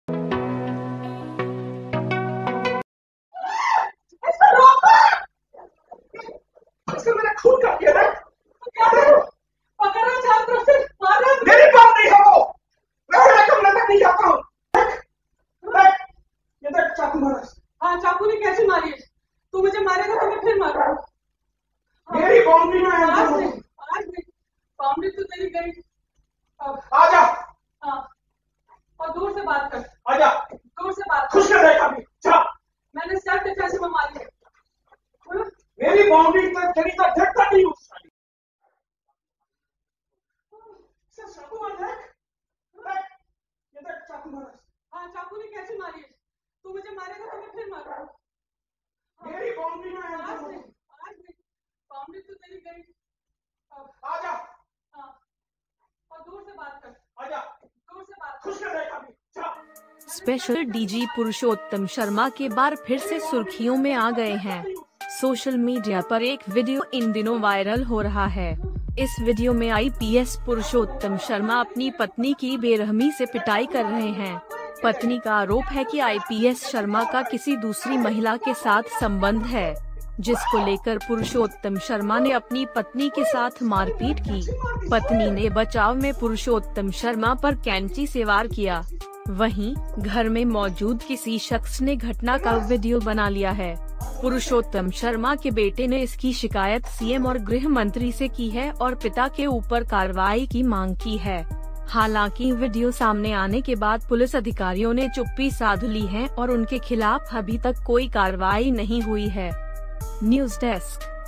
Bhopal: पत्नी की पिटाई करते हुए IPS Purushottam Sharma हुए Viral, अवैध संबंध के लगे हैं आरोप
स्पेशल डीजी पुरुषोत्तम शर्मा के बार फिर से सुर्खियों में आ गए हैं... सोशल मीडिया पर एक वीडियो इन दिनों वायरल हो रहा है... इस वीडियो में आईपीएस पुरुषोत्तम शर्मा अपनी पत्नी की बेरहमी से पिटाई कर रहे हैं...